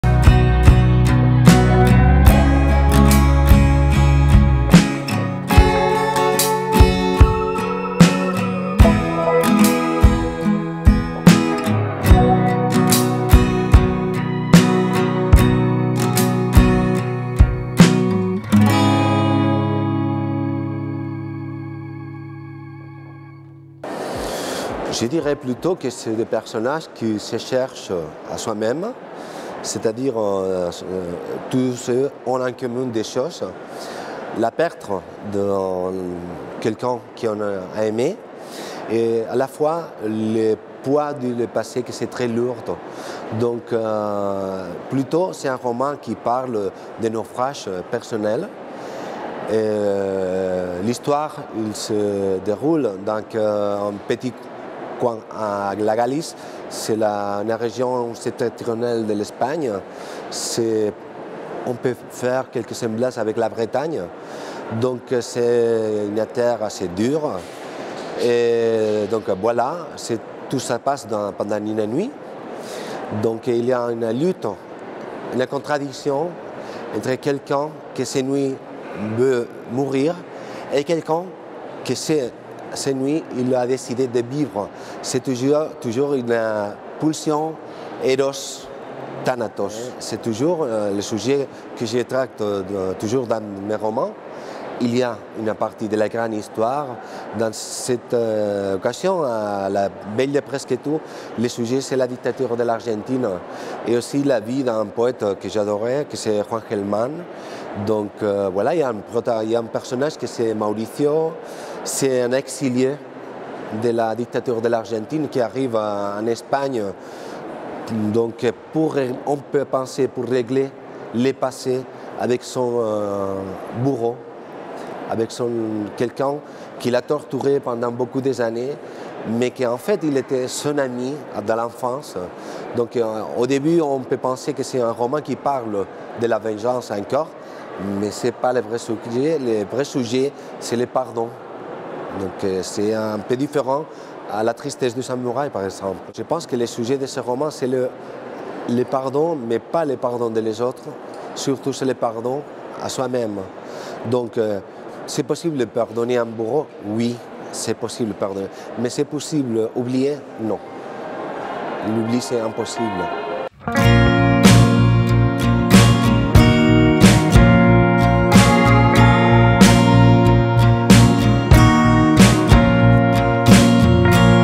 Víctor del Arbol - La veille de presque tout
À l'occasion du festival Quais du Polar à Lyon, Víctor del Arbol vous présente son ouvrage "La veille de presque tout" aux éditions Actes Sud.